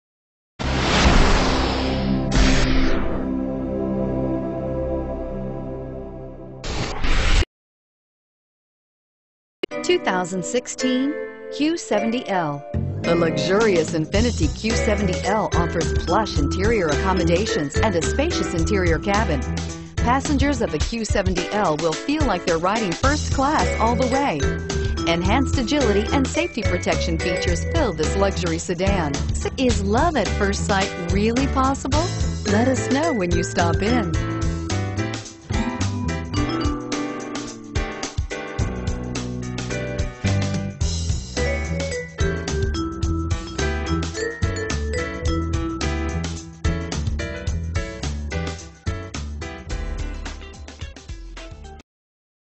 2016 Infiniti Q70L Dallas Ft. Worth Grapevine 31187
Grubbs Infiniti
1500 E State Highway 114

* 3.7 LITER 6 Cylinder engine * * GREAT DEAL AT $57,715 * * An MP3 Player ** CD Player ** Dual Climate Control ** Anti-lock Brakes ** A Backup Camera ** Traction Control ** Side Air Bag System ** Emergency Brake Assistance * * 2016 ** Infiniti * * Q70L * * 3.7 * The exterior is a beautiful black obsidian and the interior is a lovely wheat. Interested?